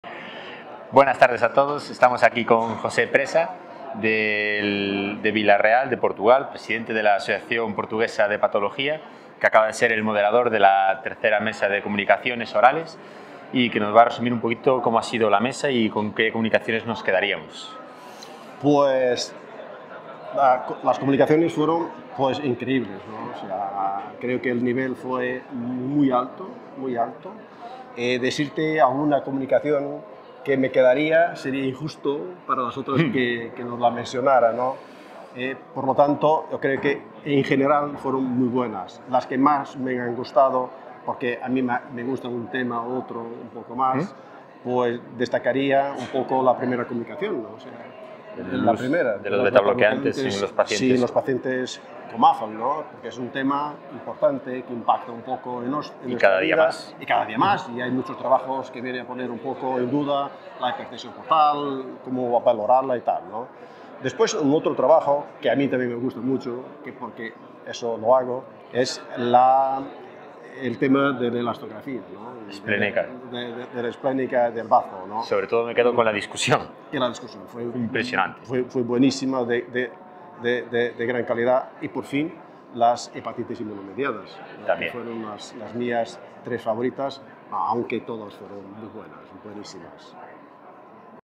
Entrevista en el pasillo. Pablo Parada entrevista a José Presa, presidente de APEF
AEEH Asociación Española para el Estudio del Hígado